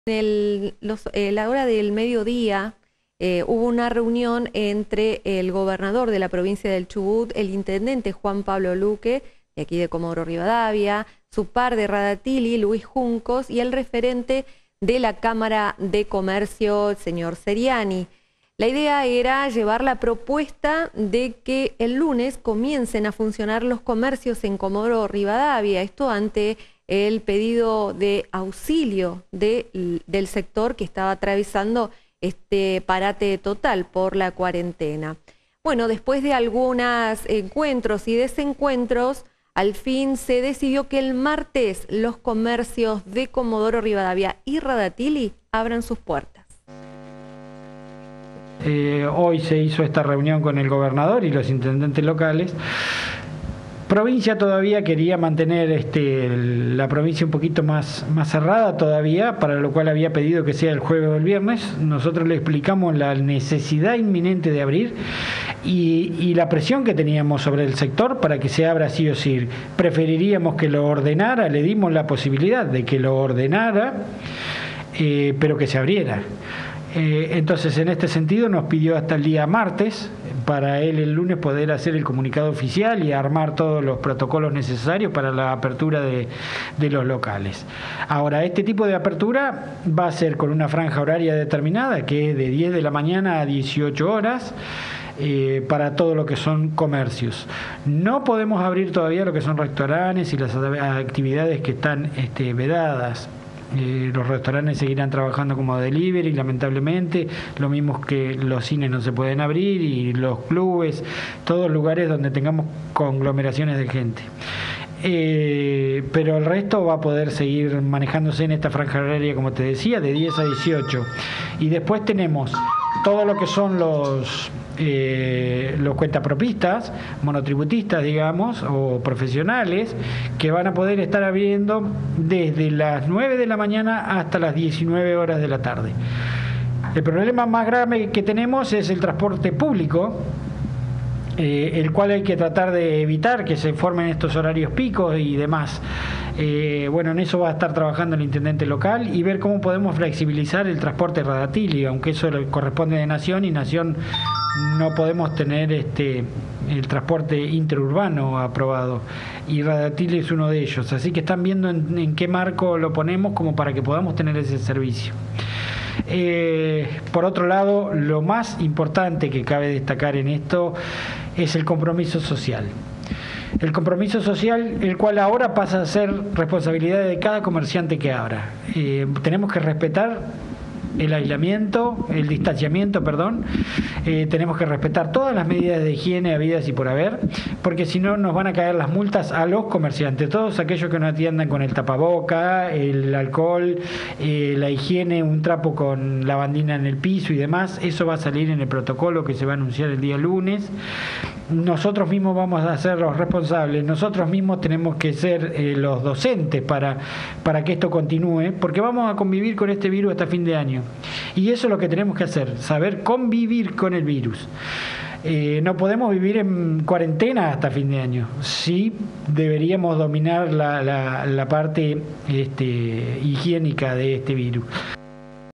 A pesar de la negativa del Gobernador, los comercios locales abriran el martes
La Cámara de Comercio de Comodoro Rivadavia solicitó una reunión con el gobernador Mariano Arcioni para plantearle junto a los intendentes de Comodoro Rivadavia, Juan Pablo Luque y Rada Tilly las necesidades de apertura de comercios minoristas para comenzar a hacer frente a las deudas y compromisos que los comerciantes deberán afrontar este mes.